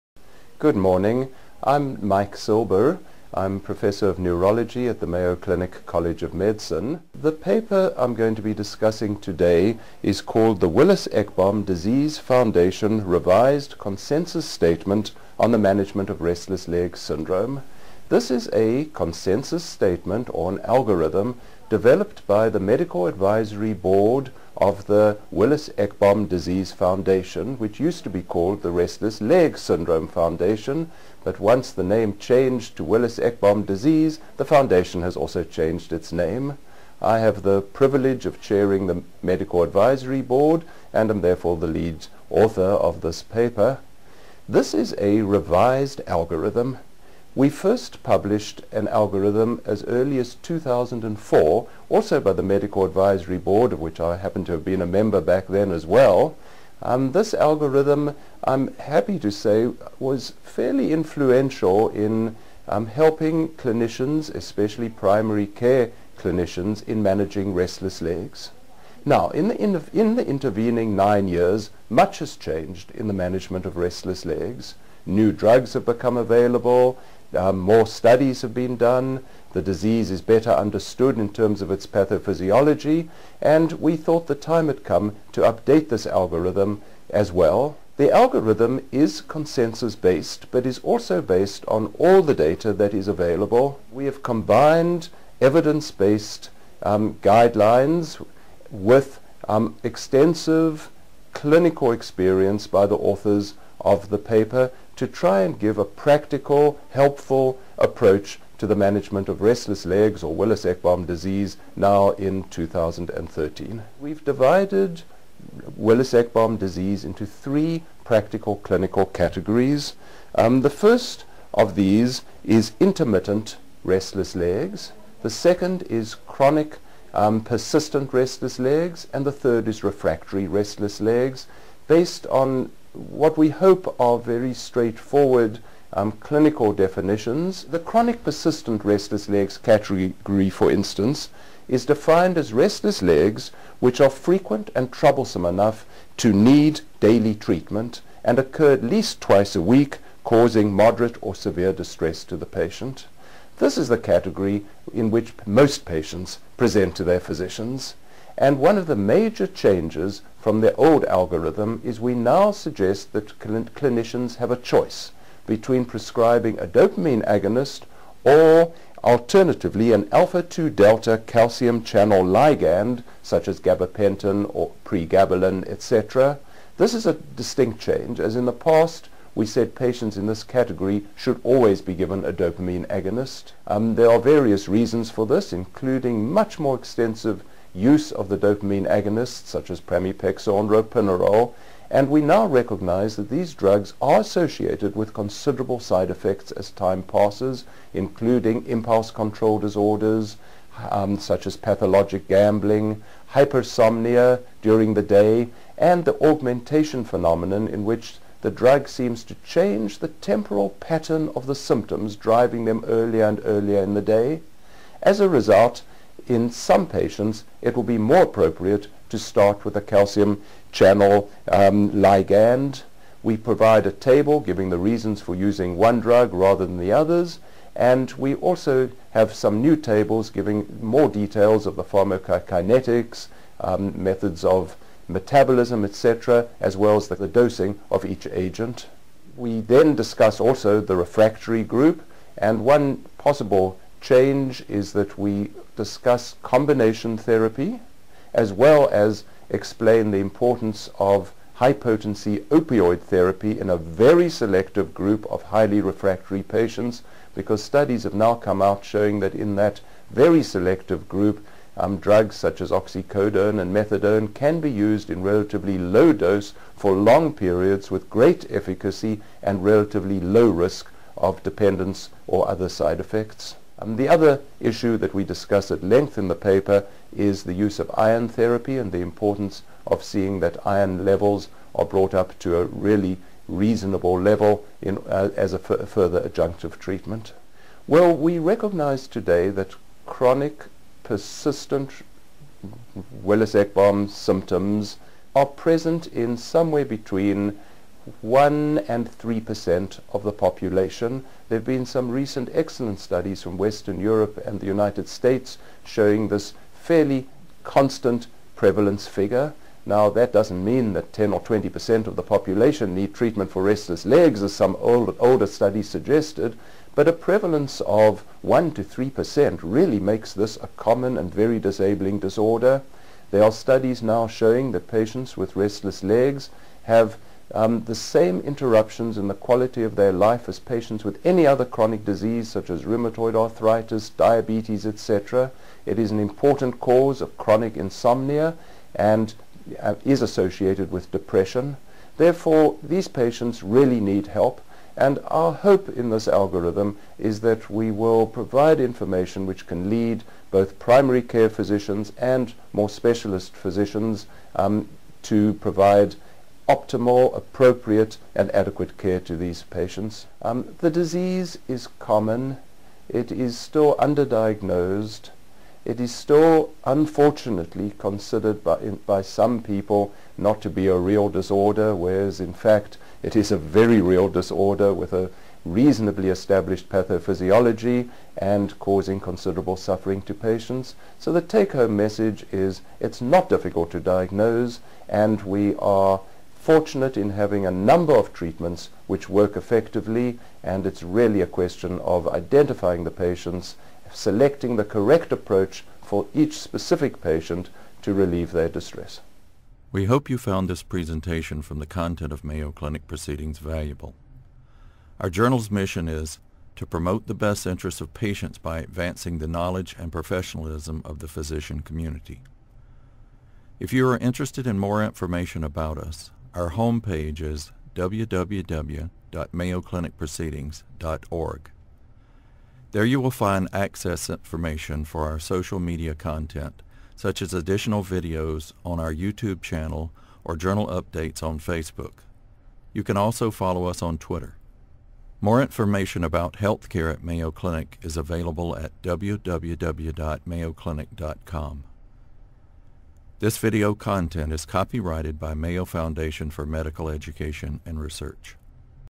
Revised Consensus Statement on Restless Legs Syndrome
Dr. Michael Silber, a Professor of Neurology at Mayo Clinic in Rochester, MN, and Chair of the Willis-Ekbom Disease Foundation Medical Advisory Board, in the September 2013 issue of Mayo Clinic Proceedings, provides an updated consensus recommendation on the management of restless legs syndrome (Willis-Ekbom disease), noting new treatment options and greater understanding for patients with this common, underdiagnosed, and often misunderstood condition.